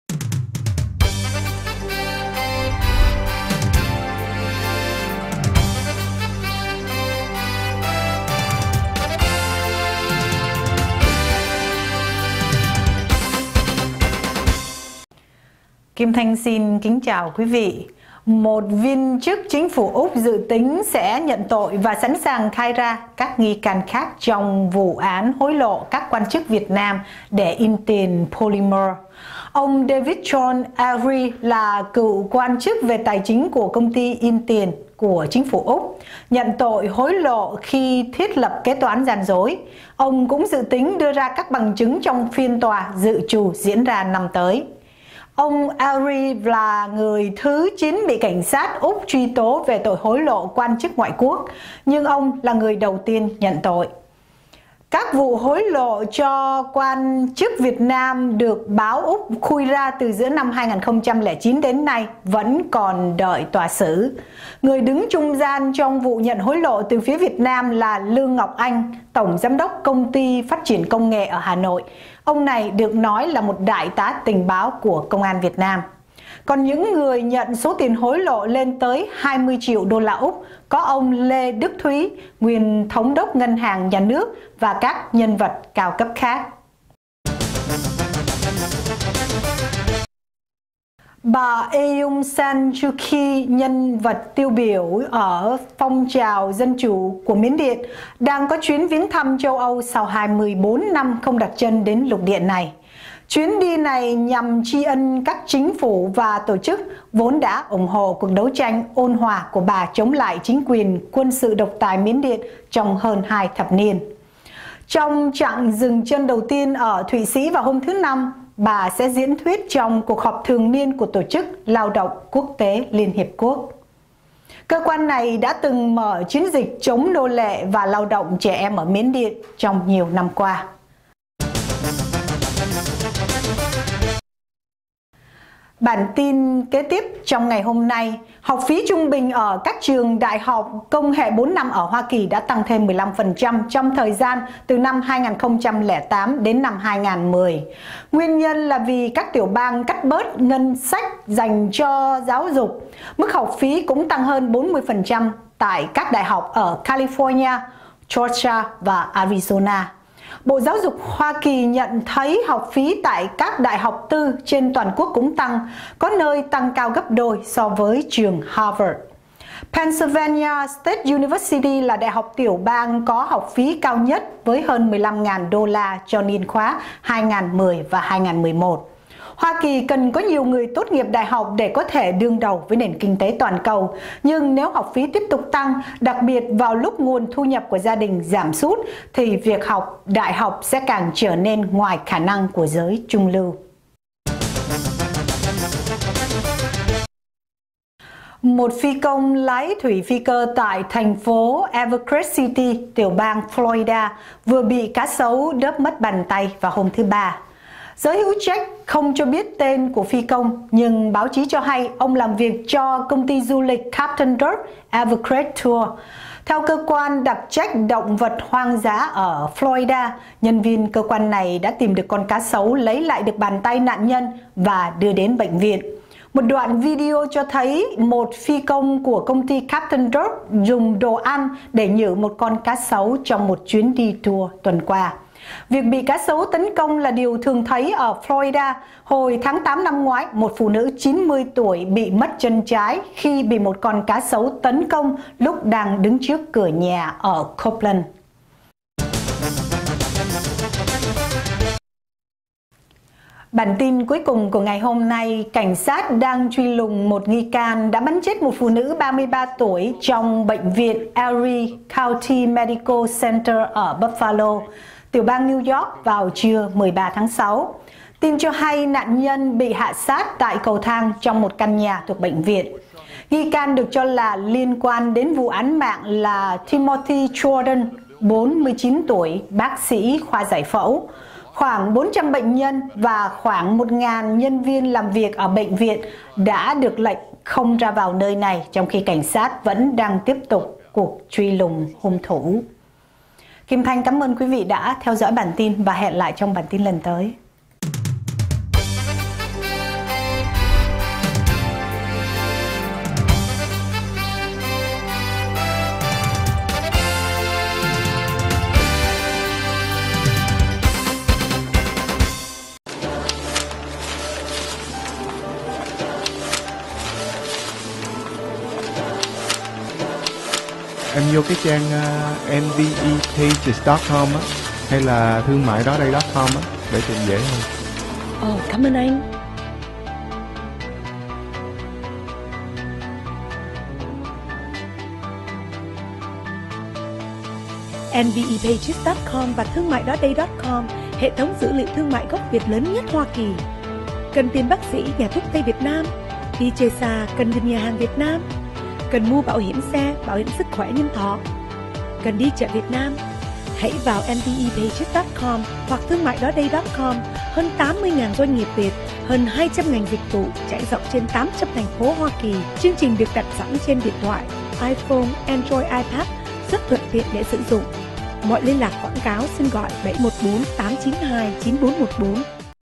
Viên chức Úc nhận tội hối lộ in tiền cho VN.
Một viên chức chính phủ Úc dự tính sẽ nhận tội và sẵn sàng khai ra các nghi can khác trong vụ án hối lộ các quan chức Việt Nam để in tiền polymer.

Ông David John Ellery là cựu quan chức về tài chính của công ty in tiền của chính phủ Úc, nhận tội hối lộ khi thiết lập kế toán gian dối. Ông cũng dự tính đưa ra các bằng chứng trong phiên tòa dự trù diễn ra năm tới.

Ông Ellery là người thứ 9 bị Cảnh sát Úc truy tố về tội hối lộ quan chức ngoại quốc, nhưng ông là người đầu tiên nhận tội.